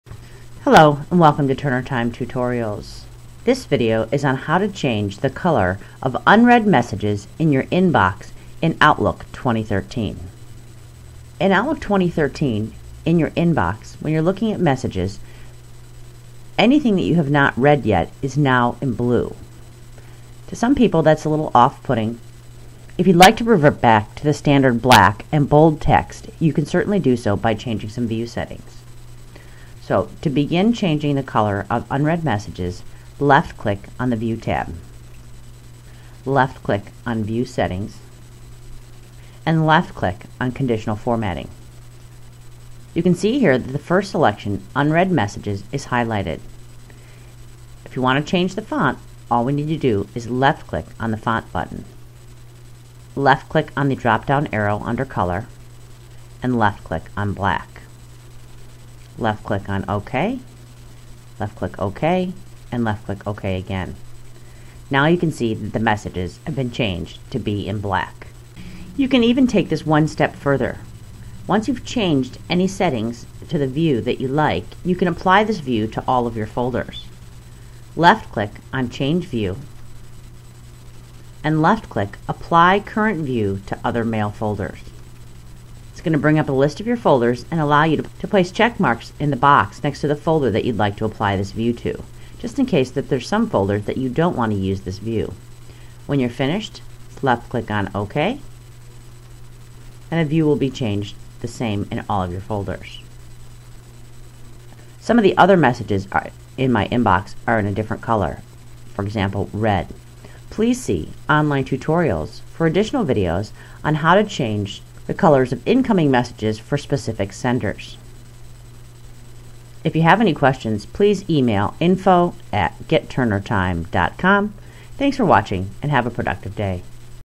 How to Change Color of Unread Messages In Inbox (Outlook 2013) - by Turner Time Management
In Outlook 2013, new, unread messages are in a bold blue color in your inbox.  If you want to change the color back to black it can be done using "View Setting" and "Conditional Formatting".